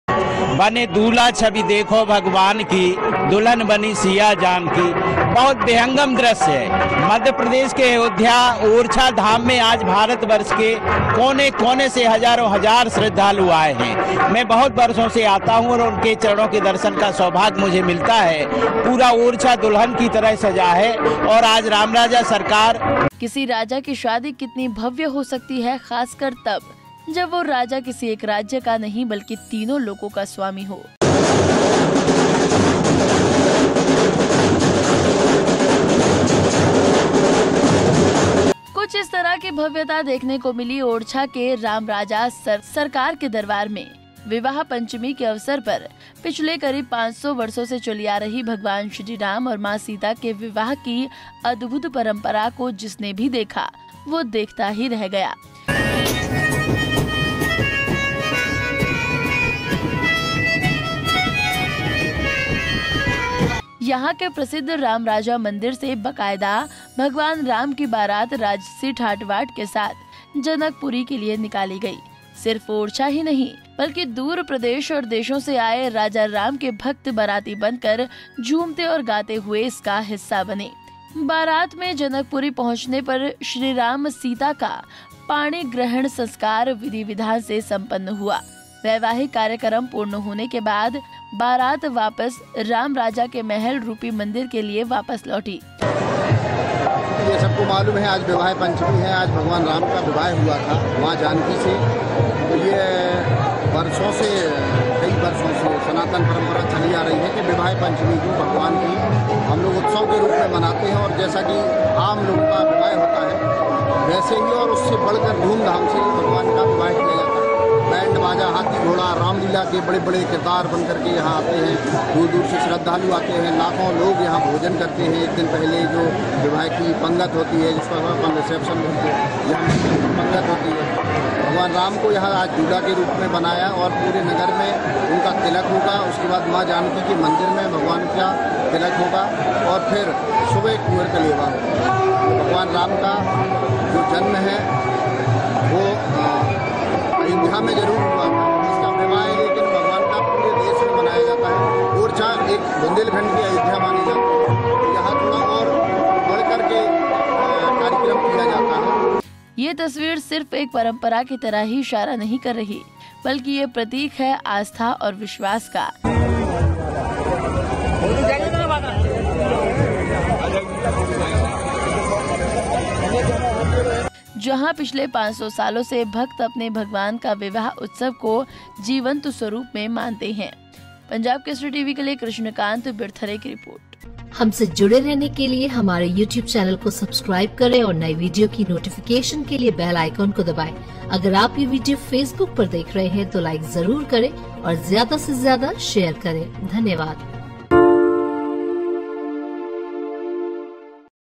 मैया जानकी के घर बारात लेकर पहुंचे राजाराम! फिर तीन लोक के स्वामी को जनक ने सौंपी अपनी पुत्री,
मैया जानकी के घर बारात लेकर पहुंचे राजाराम! फिर तीन लोक के स्वामी को जनक ने सौंपी अपनी पुत्री, बुंदेलखंड की अयोध्या में अद्भुत आयोजन...

Also Watch ►